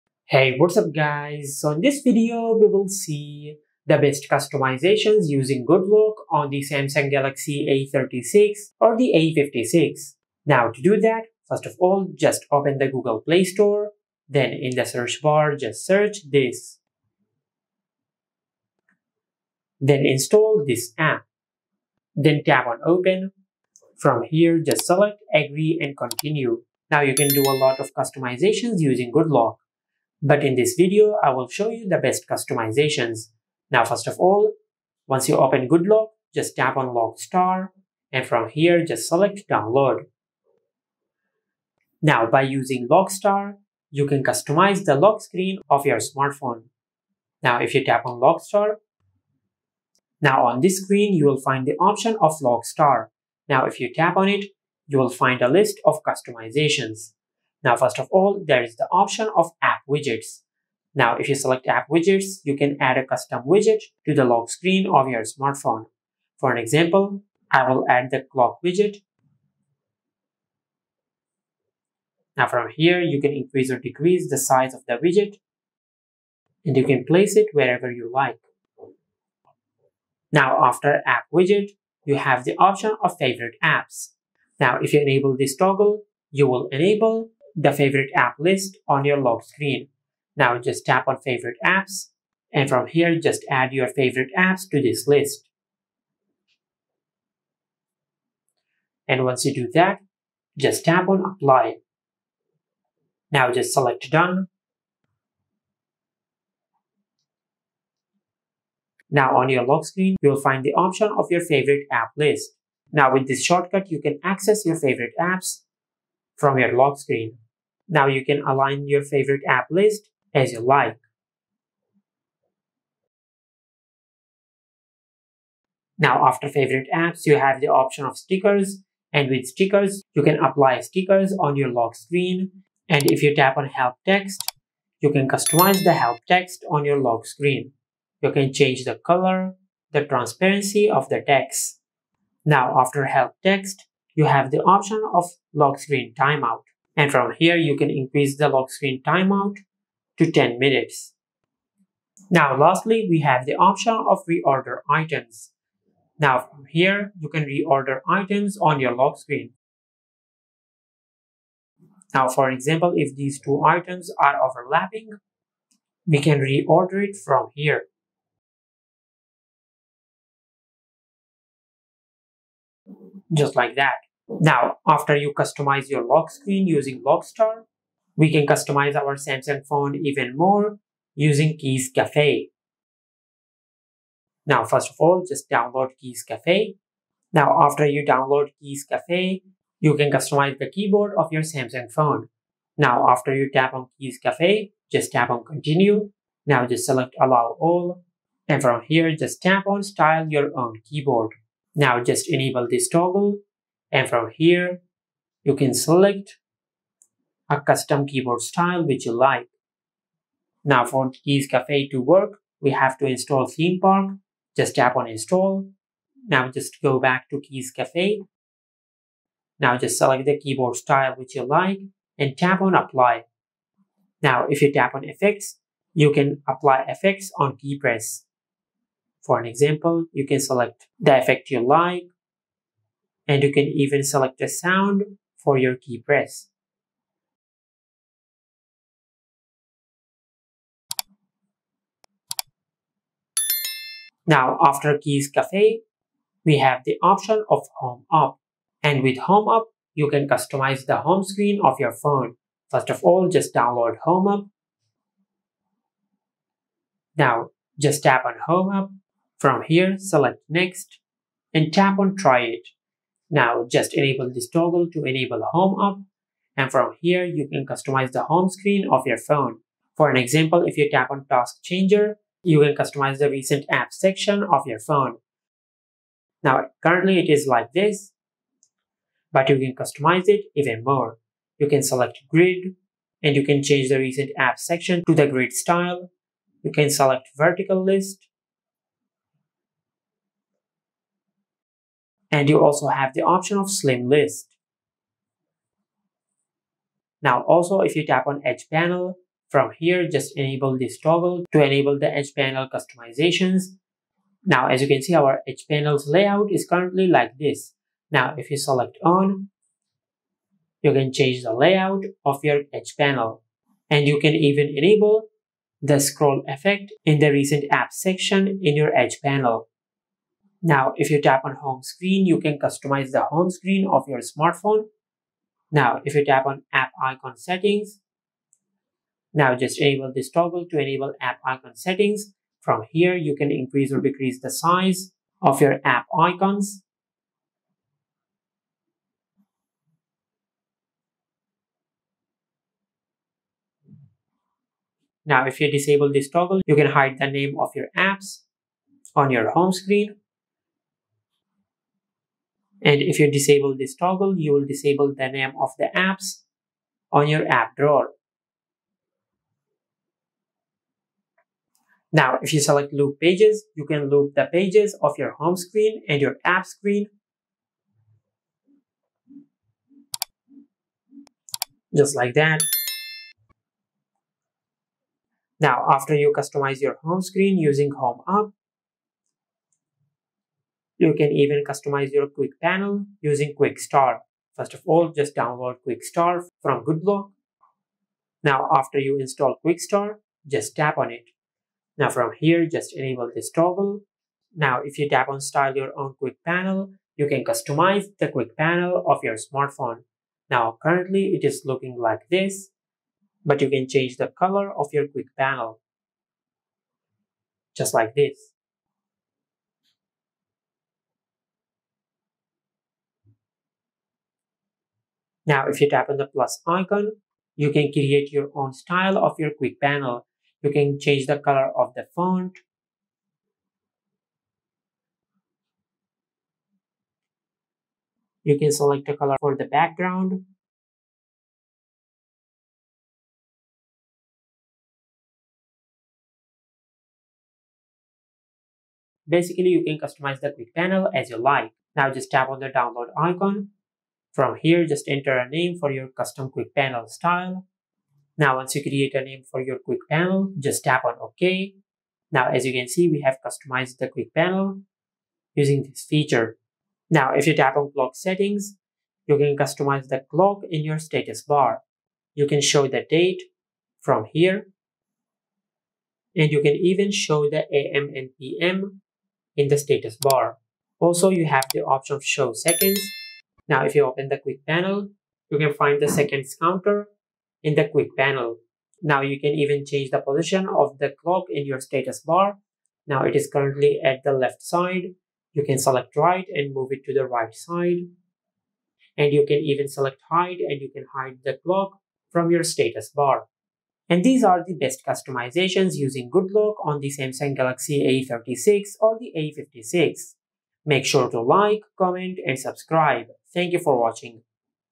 Best Customization Using Good Lock in Samsung A36/A56
In this video, we will learn the best customization options using Good Lock on Samsung A36/A56. If you want to change themes, tweak UI settings, or unlock hidden customization features on Samsung A36/A56, follow this guide.